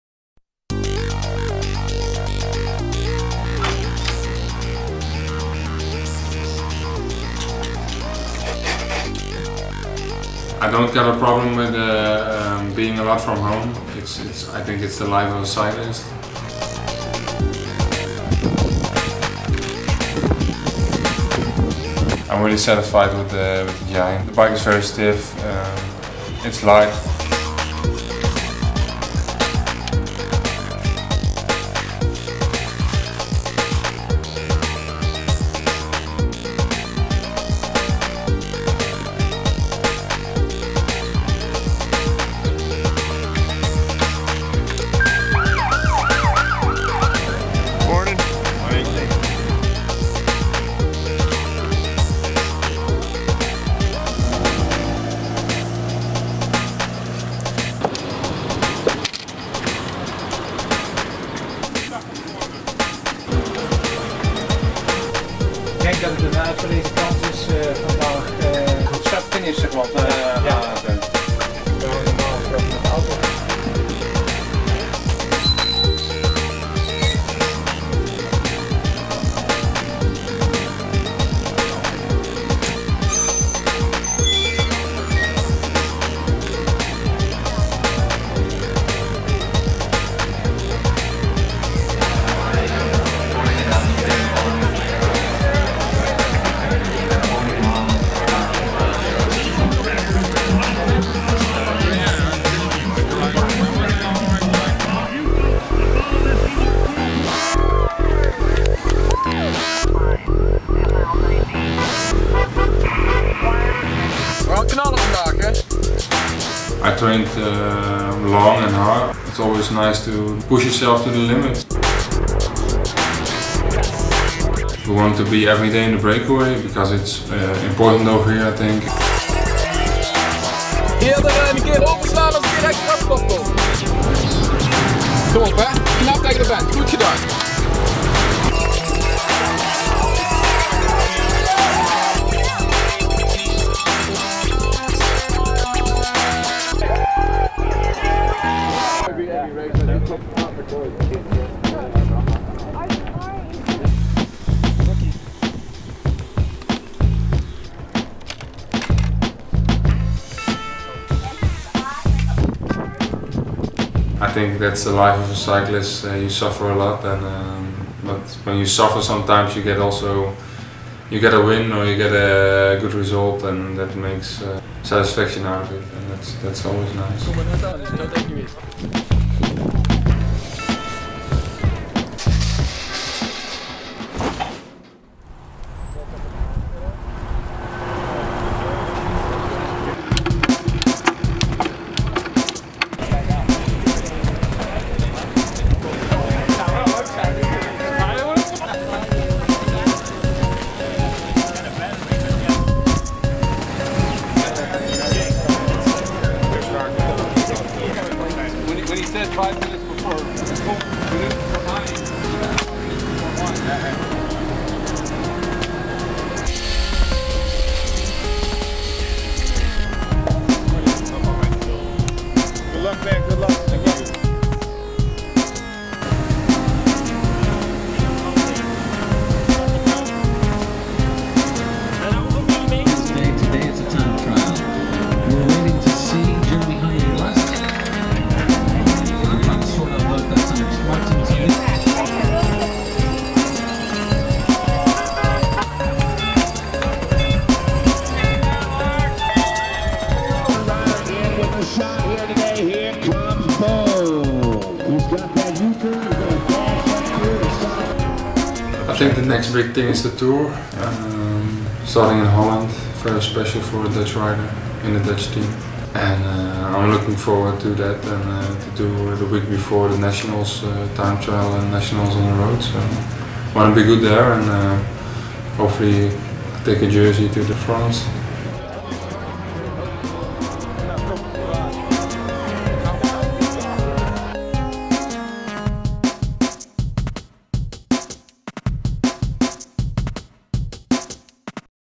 RIDE LIFE: RABOBANK'S LARS BOOM
Experience the life of a pro cyclist as you follow Rabobank Team rider Lars Boom over the course of three days in the 2010 Amgen Tour of California. Videographer Jake Orness captures the essence of what it takes to race at the highest level.  Music by Giant staffer Jeremy Combs.